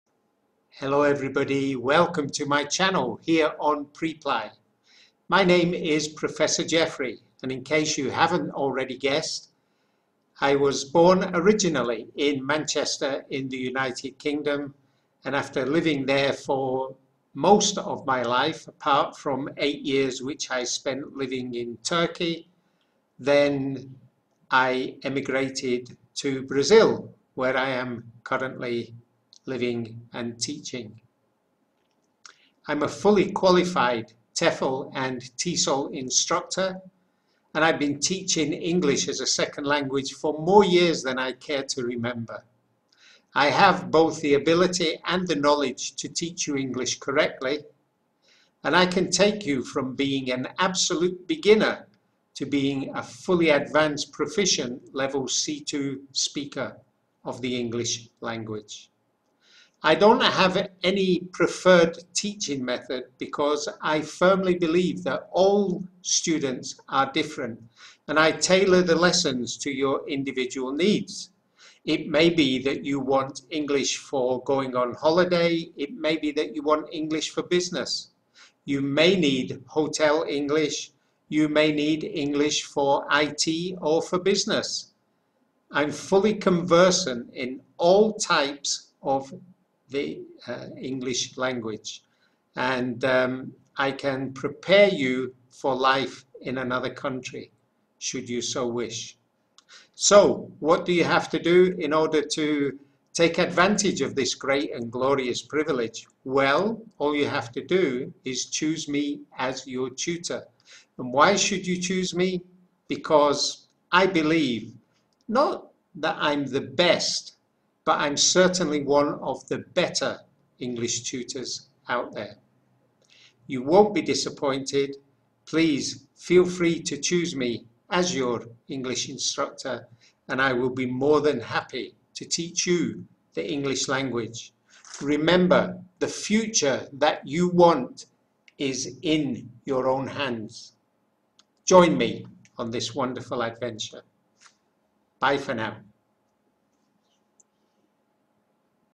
An introduction by Professor Geoffrey
How to learn English the easy way with Prof. Geoffrey.
Aulas de inglês online, individual ou em grupo com um professor de inglês britânico. Os certificados são válidos em todo o mundo.